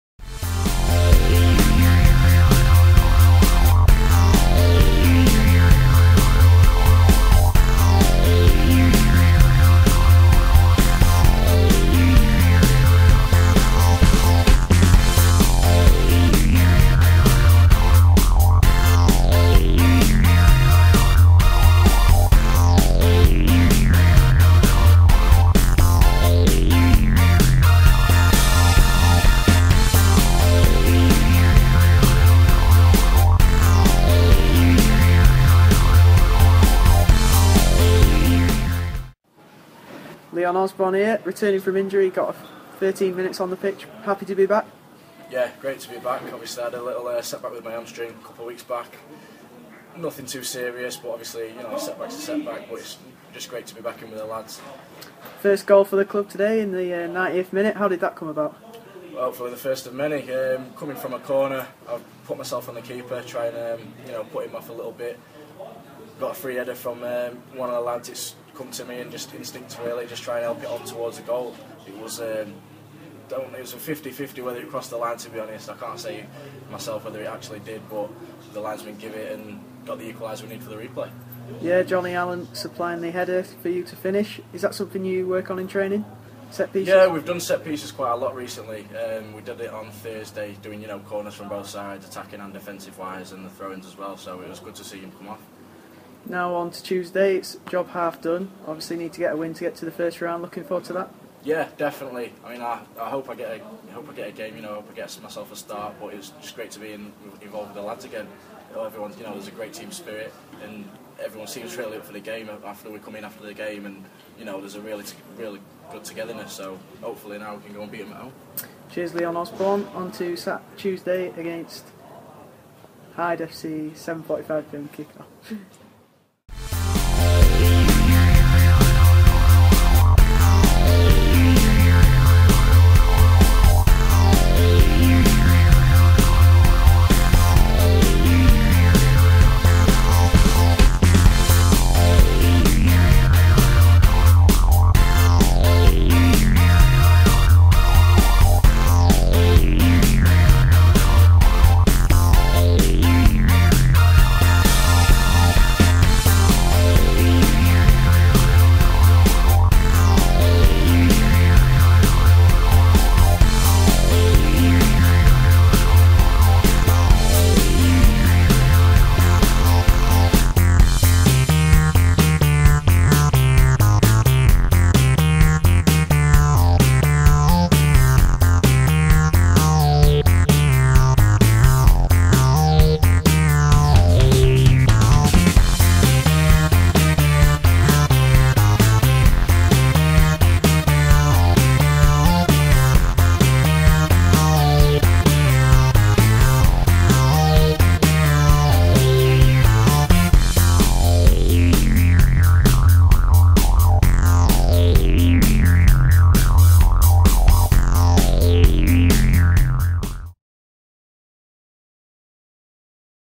Leon Osborne Interview 201012.wmv
Goalscorer Leon Osborne talks to Jonny Lally following the 1-1 draw against Hyde FC in the FA Cup 4th Qualifying Round.